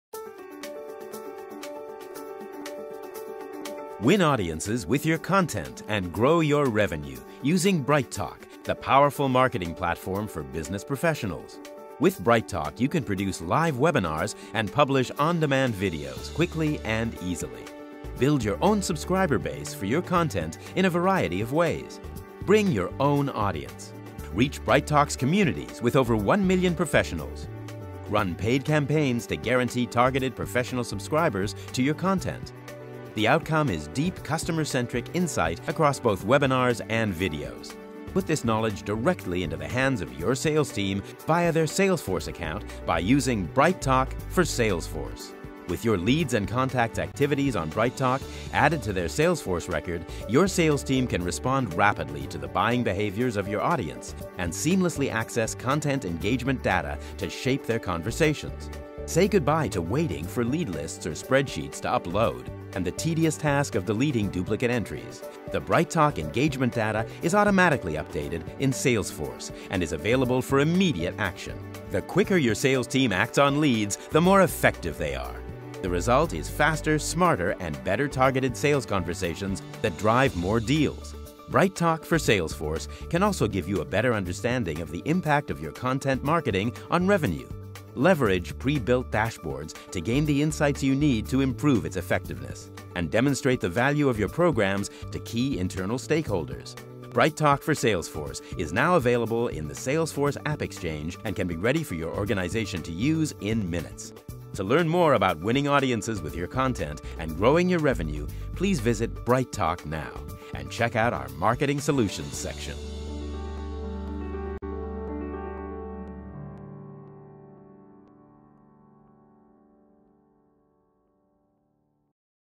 BrightTALK for Salesforce
BrightTALK for Salesforce enables your sales team to respond rapidly to the buying behaviors of your audience by offering real-time access to your BrightTALK engagement data.

You can get BrightTALK for Salesforce from the Salesforce AppExchange.